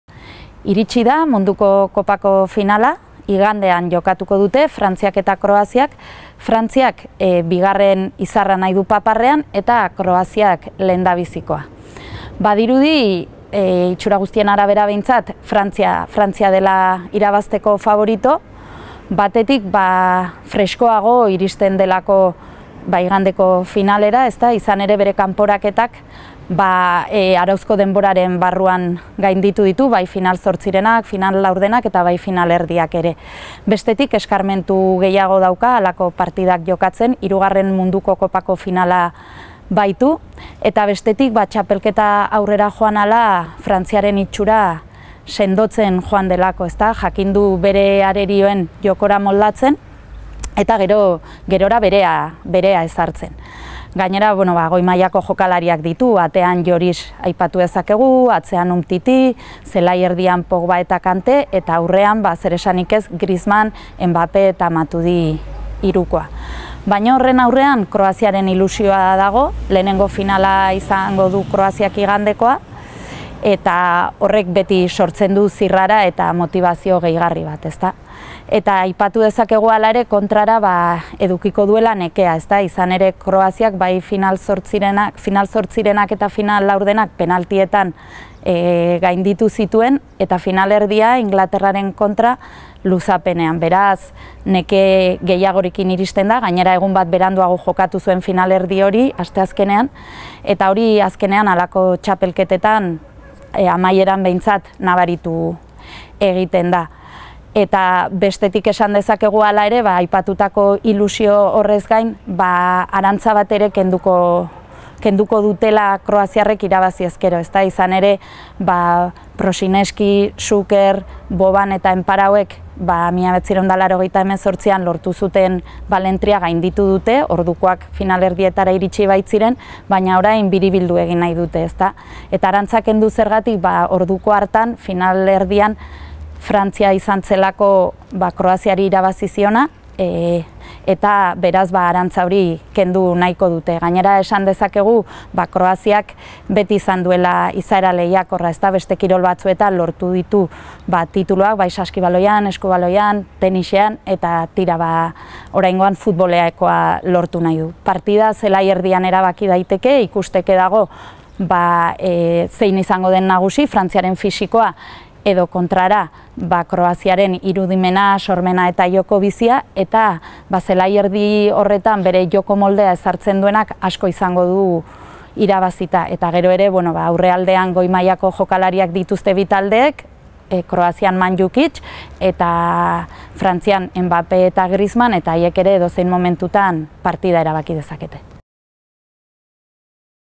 Ainara Arratibelen analisia, finalaren atarian
| Futboleko Munduko Kopako finala jokatuko dute igande honetan Frantziak eta Kroaziak. Halako partidetan duen esperientziagatik eta txapelketan erakutsi duen nagusitasunagatik, Frantzia da faborito, Ainara Arratibel kazetariaren arabera.